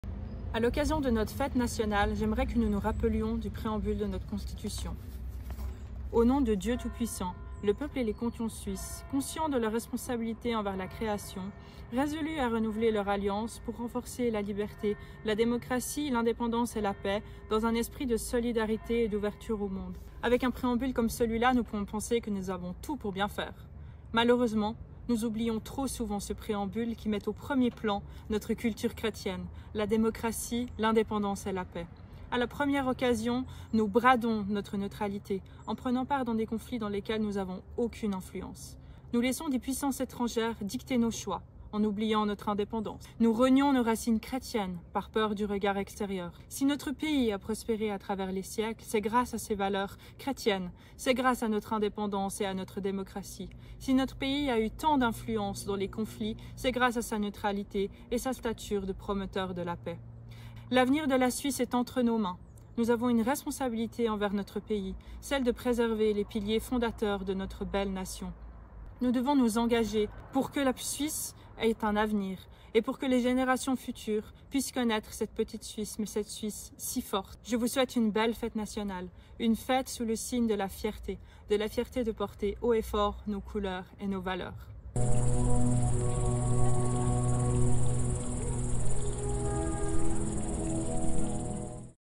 Pro Suisse vous souhaite à tous un 1er août recueilli, digne et convivial.
Pour Pro Suisse, il est primordial de motiver également les jeunes Suisses à s'engager pour leur patrie. Pour cette raison, des jeunes du Tessin, de Suisse romande et de Suisse alémanique auront également l'occasion de s'exprimer par le biais d'un court message.

Emmylou Ziehli-Maillard